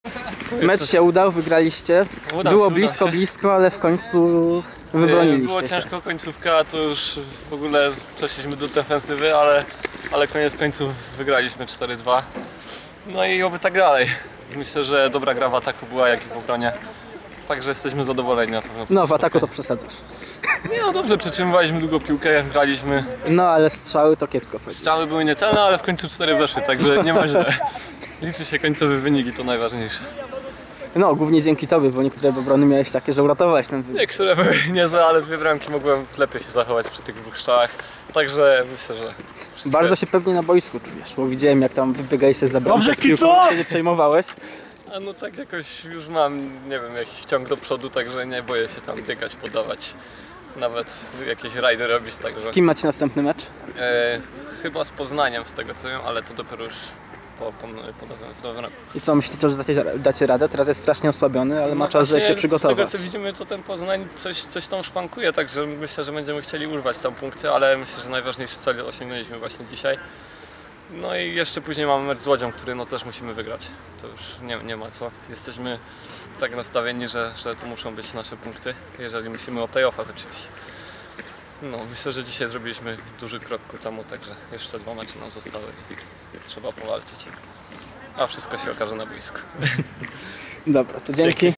Wypowiedź pomeczowa: Dawid Zając
Bramkarz Spartans Oświęcim w rozmowie o meczu derbowym z Kraków Lacrosse Kings